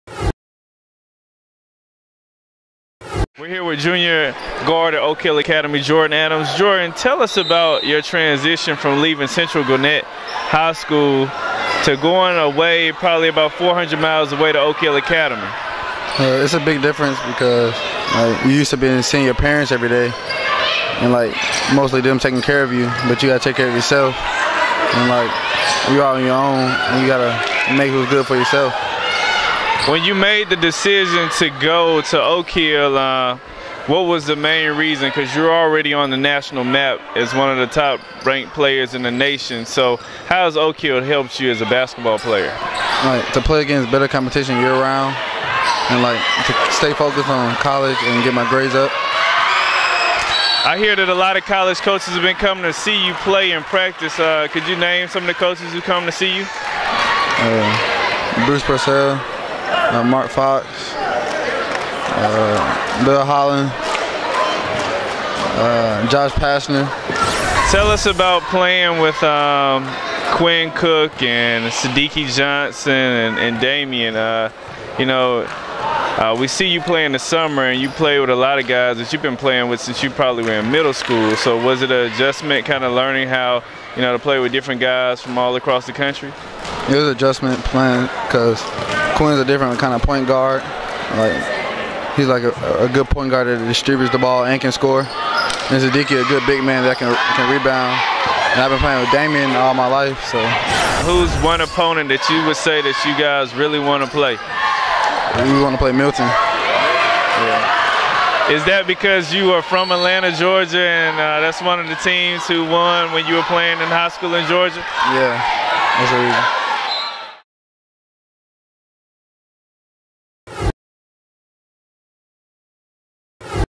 Oak Hill Academy's Jordan Adams talks about transition
Jordan Adams talks about his transition from leaving Central Gwinnett to attend Oak Hill Academy.